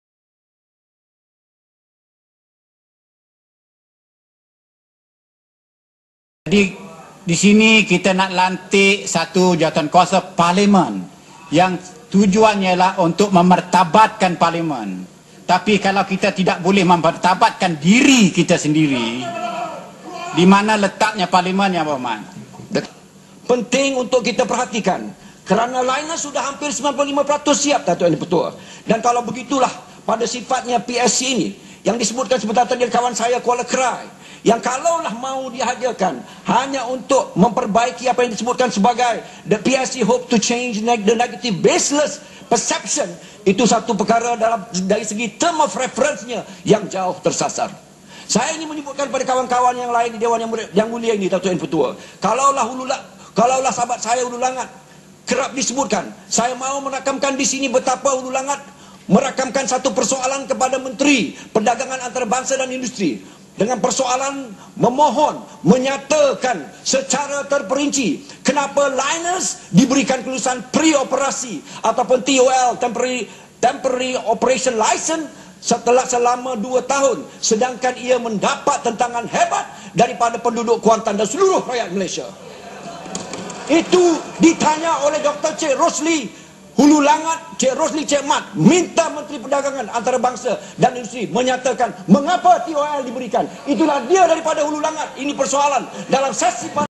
Terms of Reference, PSC for LAMP, Lynas Project.
A debate on the term of reference of the Parliament Select Commitee for LAMP, the Lynas Project in Gebeng Kuantan.